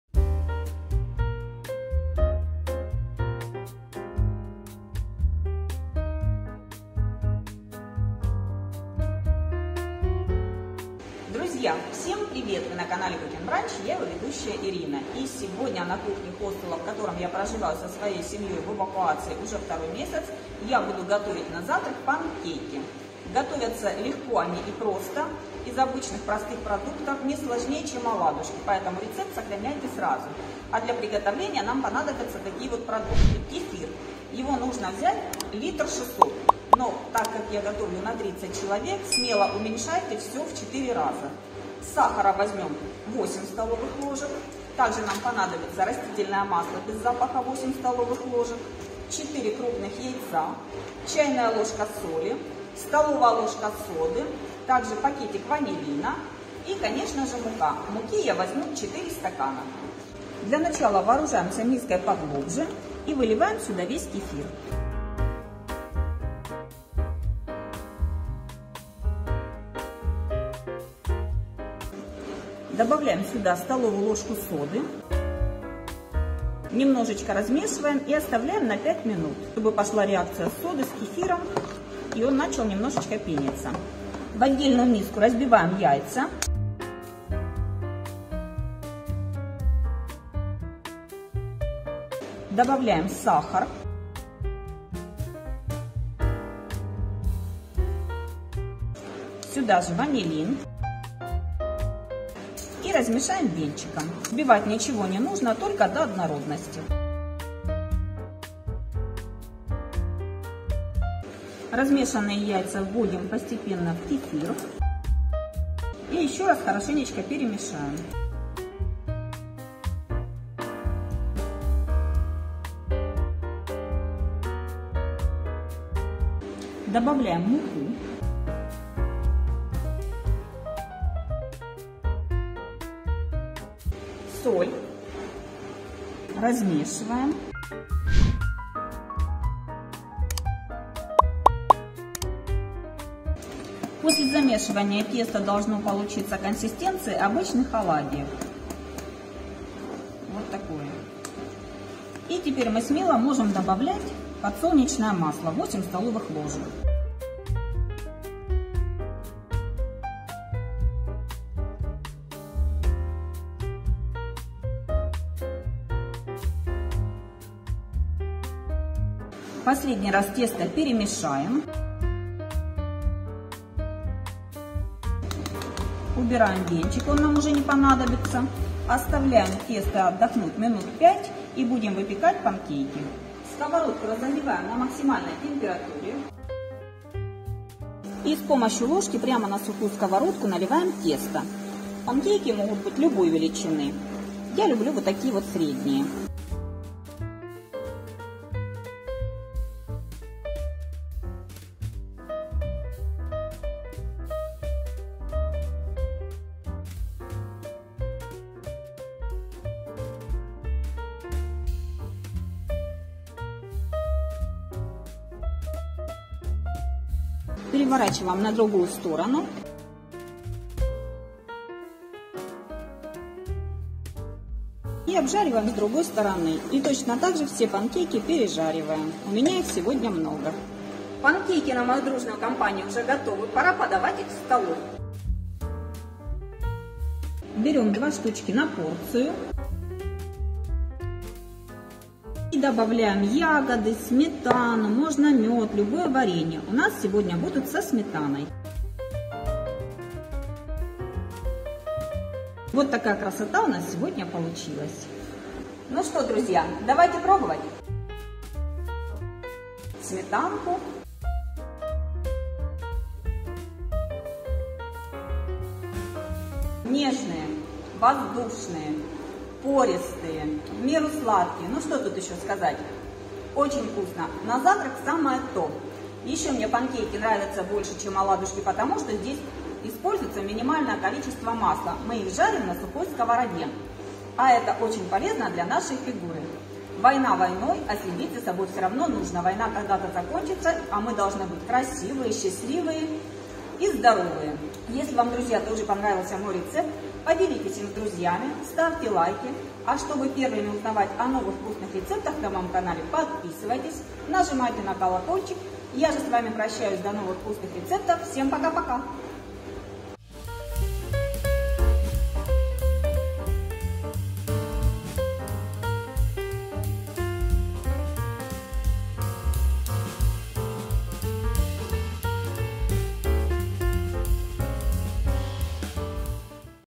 Харьков. Эвакуация. Готовлю завтрак на 30 человек | Пышные панкейки на кефире | Проверенный рецепт
Дорогие друзья! В Украине война! Сейчас я со своей семьей нахожусь в эвакуации. И чтоб хоть немного отвлечься, я занимаюсь своим любимым делом. Сегодня я готовлю завтрак на 30 человек. Приготовим пышные панкейки на кефире. Проверенный рецепт!!! Готовьте с удовольствием!

РЕЦЕПТ:
Кефир - 1,6 литра
Сахар - 8 ст. ложек
Мука пшеничная - 4 стакана
Масло растительное - 8 ст. ложек
Яйца куриные - 4 шт
Ванилин - 1 пакетик
Сода - 1 ст. ложка
Соль - 1 ч. ложка

RECIPE:
Kefir - 1.6 liters
Sugar - 8 tbsp. spoons
Wheat flour - 4 cups
Vegetable oil - 8 tbsp. spoons
Chicken eggs - 4 pcs
Vanillin - 1 sachet
Soda - 1 tbsp. a spoon

Дорогие друзья, пишите в комментариях на сколько интересен вам ролик. Задавайте интересующие вас вопросы.